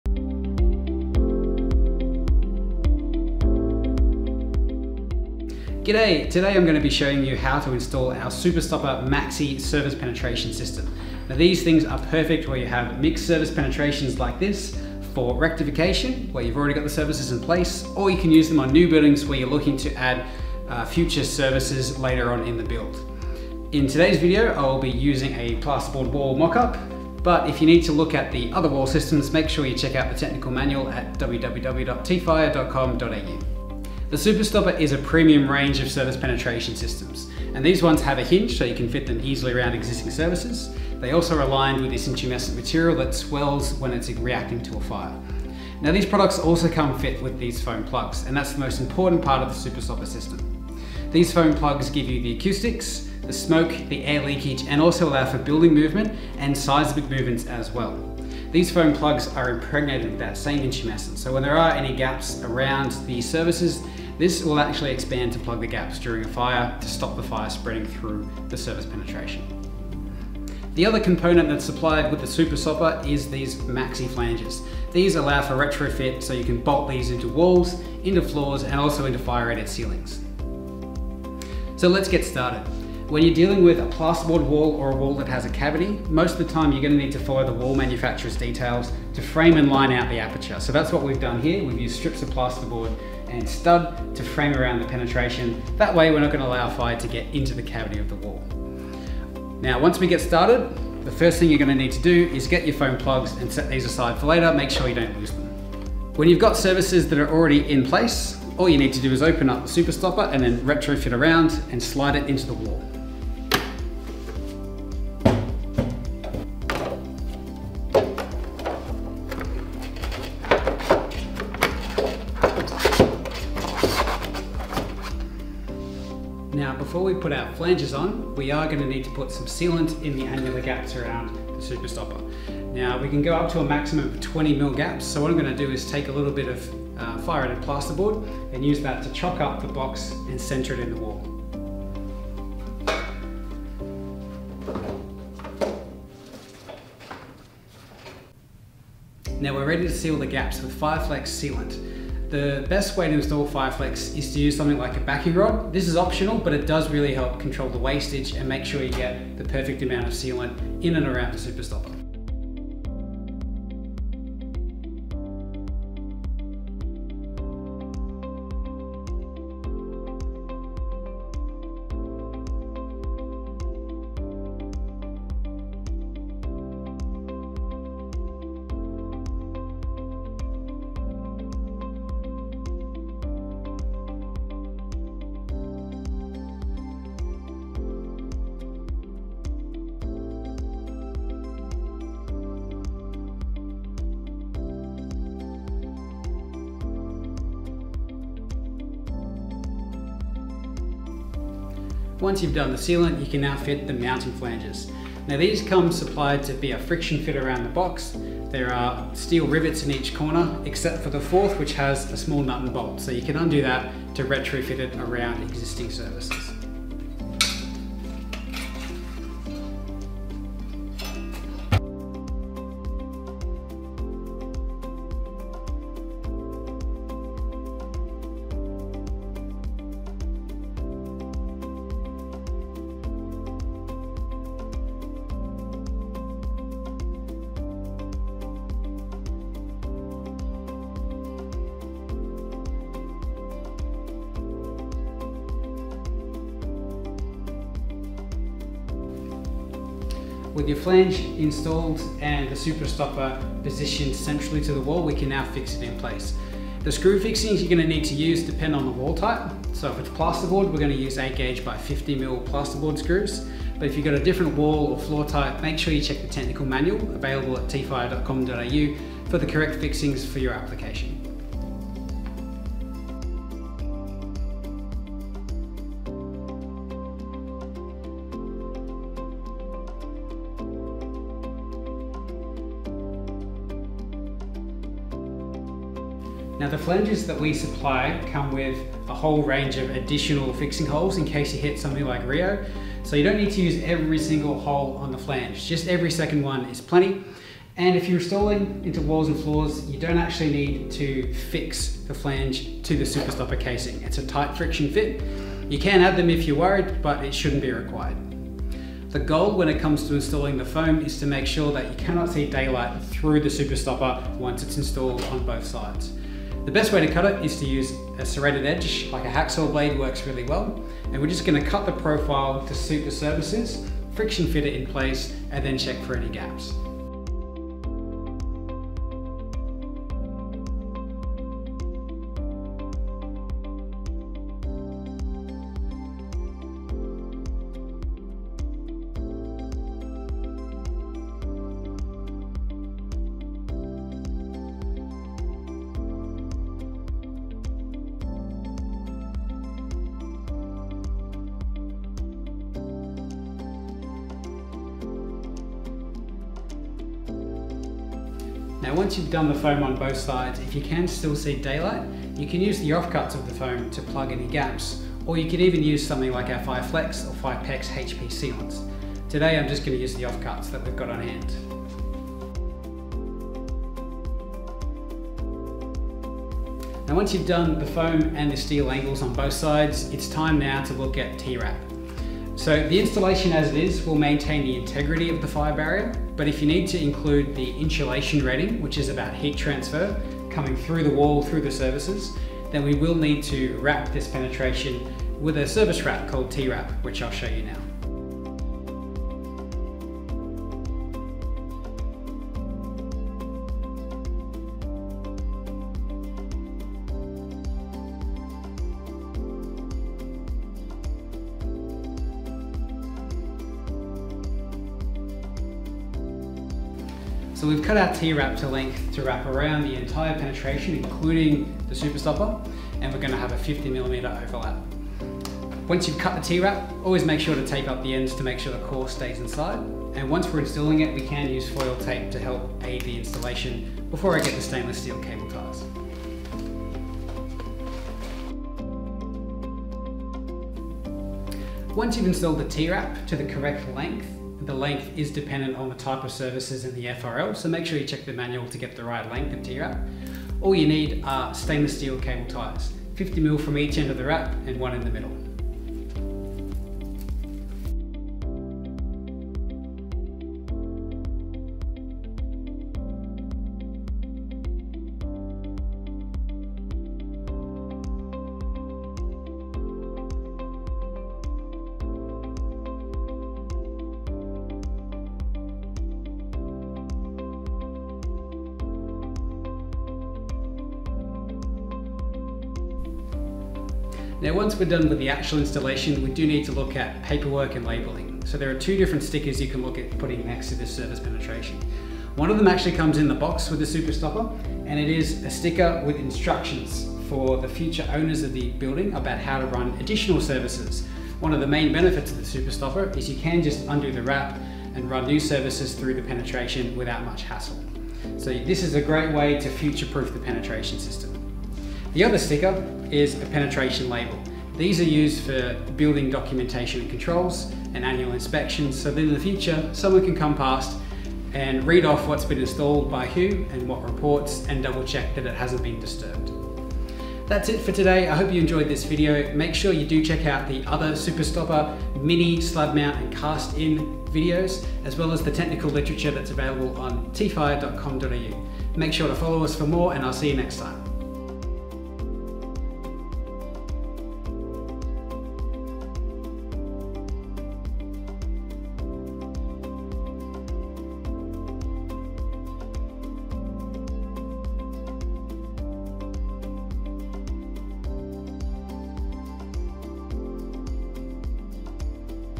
SuperSTOPPER® Maxi Installation Video
In this video, learn how to install the SuperSTOPPER® Maxi, a versatile fire protection system designed to safeguard multiple and mixed services passing through concrete slabs, walls, and ceilings.

Key Features:

- Allows multiple and mixed services
- Eliminates the need for a 200mm separation between adjacent services
- Suitable for both retrofit applications and new builds
- Australia's most tested fire stopping system
-Intumescent foam plugs with a variety of fire-stopping benefits